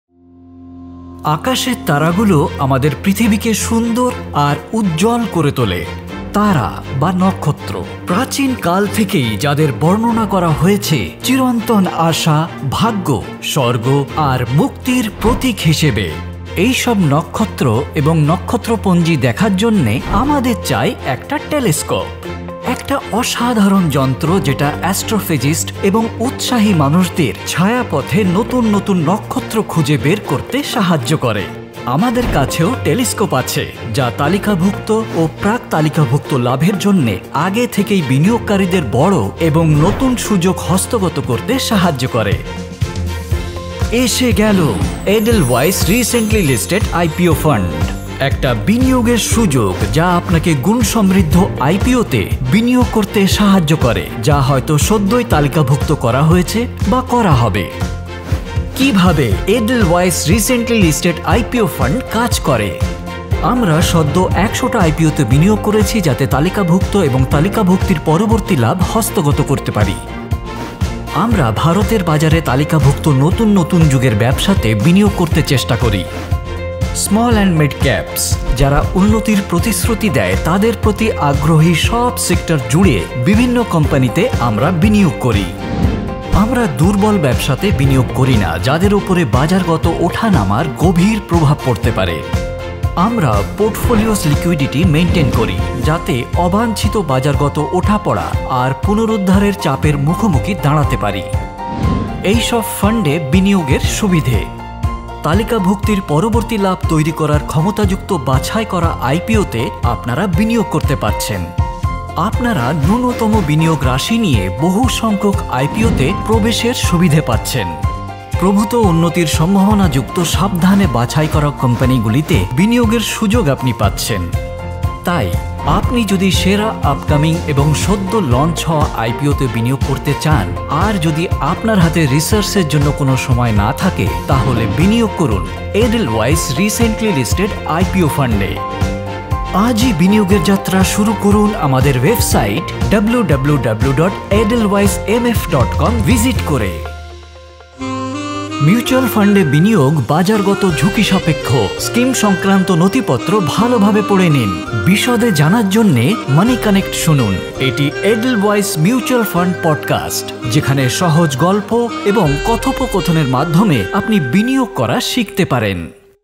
Invest in IPOs of emerging Indian companies with the Edelweiss Recently Listed IPO Fund (Bengali)
Presenting the Edelweiss Recently Listed IPO Fund that will enable you to invest in quality IPOs, that are either recently listed, or are going to get listed.

The Edelweiss Recently Listed IPO Fund invests in the recent 100 IPOs to capture listing and post-listing gains. It tries to invest in new age businesses that are getting listed in Indian Market. The investment is done across
sectors with a bias towards small and mid-caps that promise growth.

The fund avoids investing in weak businesses that can be highly impacted by market shocks.

Benefits of Investing in Edelweiss Recently Listed IPO Fund:
• You get to invest in select IPOs with potential to generate post-listing gains
• You get access to a large number of IPOs with minimal investment amount
• You get to invest in carefully selected companies that have the potential to grow large

0:00 – Introduction
0:41 – Presenting the Edelweiss Recently Listed IPO Fund
0:54 – How Does Edelweiss Recently Listed IPO Fund Work
1:34 – Benefits of Investing in Edelweiss Recently Listed IPO Fund